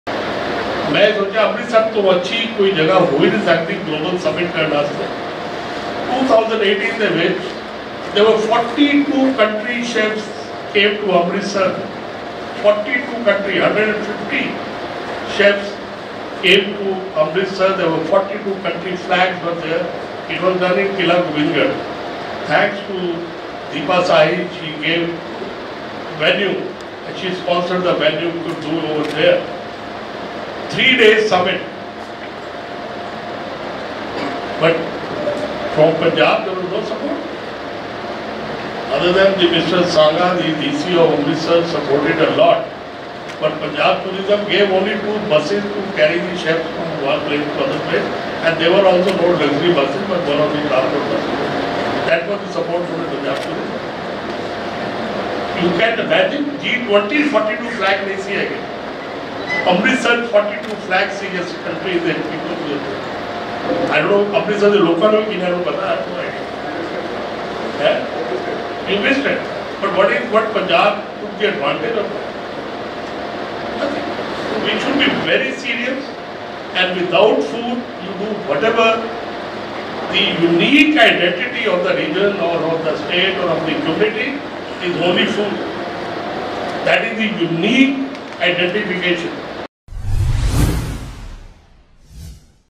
42 ਮੁਲਕਾਂ ਦੇ ਚੋਟੀ ਦੇ Chef ਆਏ ਅੰਮ੍ਰਿਤਸਰ -ਨਾ ਸਰਕਾਰ ਕੋਈ ਮੱਦਦ ਕੀਤੀ ਨਾ ਲਿਆ ਕੋਈ ਲਾਹਾ
42 ਮੁਲਕਾਂ ਦੇ ਚੋਟੀ ਦੇ Chef ਆਏ ਅੰਮ੍ਰਿਤਸਰ -ਨਾ ਸਰਕਾਰ ਕੋਈ ਮੱਦਦ ਕੀਤੀ ਨਾ ਲਿਆ ਕੋਈ ਲਾਹਾ